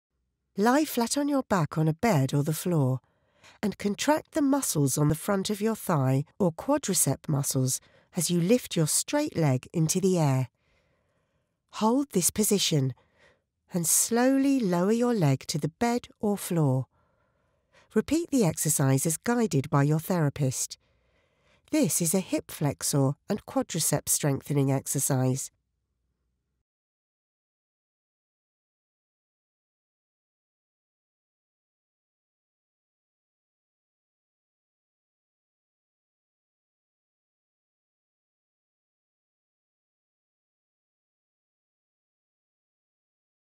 Straight Leg Raise Quad Strength
Lie flat on your back on a bed or the floor, and contract the muscles on the front of your thigh, or quadricep muscles, as you lift your straight leg into the air. Hold this position, and slowly lower your leg to the bed or floor. Repeat the exercise as guided by your therapist. This is a hip flexor and quadricep strengthening exercise.